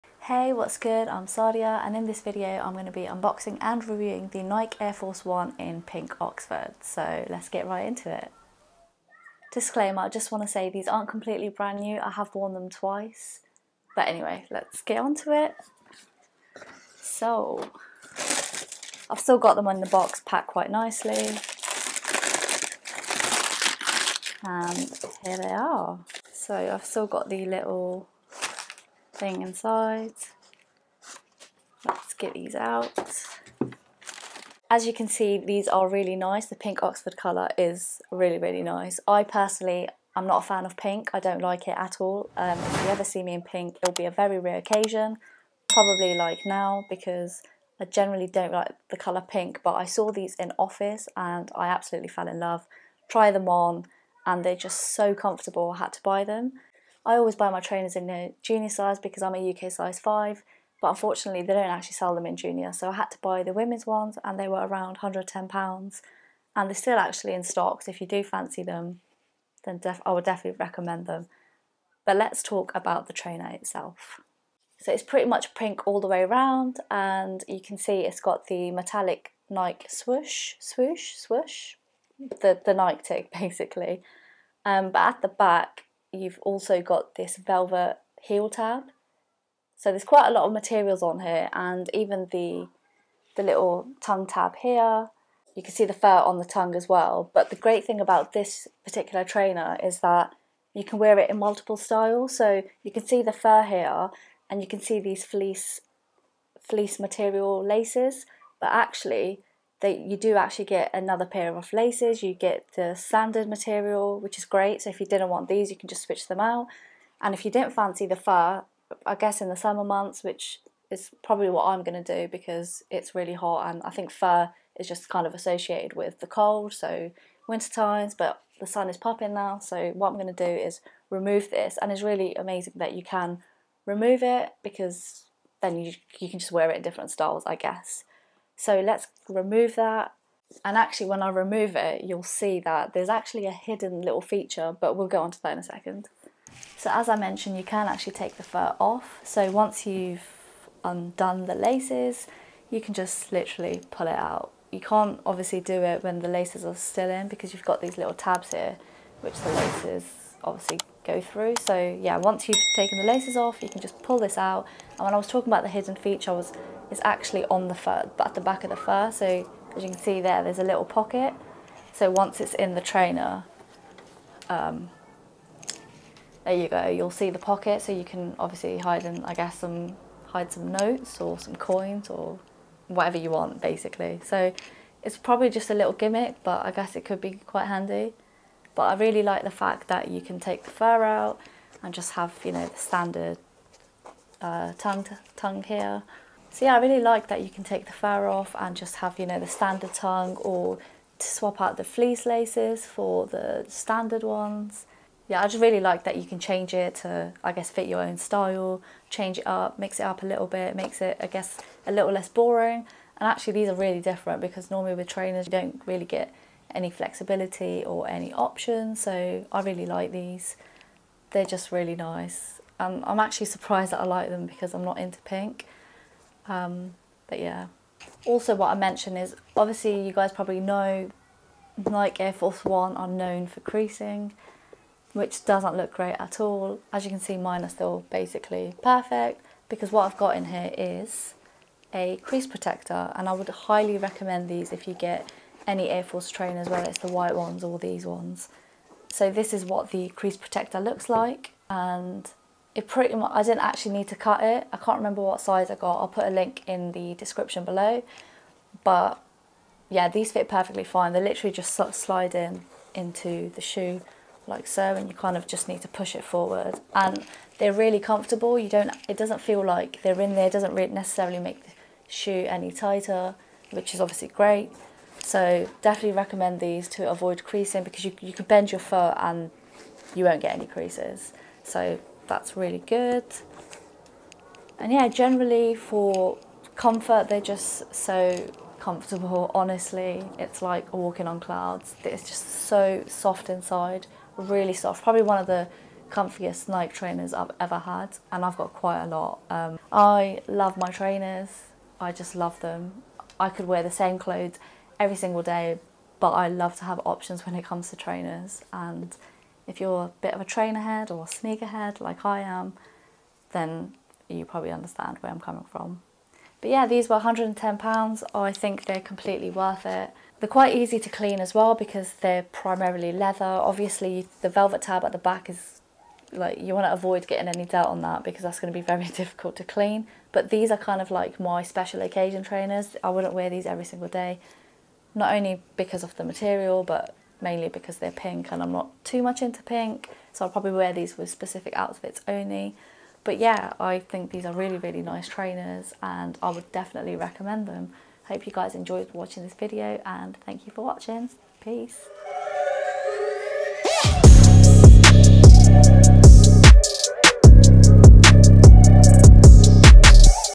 Nike Air Force 1 Pink Oxford | Unboxing & Review
Sadia provides an unboxing & review of the Nike Air Force 1 Pink Oxford trainers

Crease protectors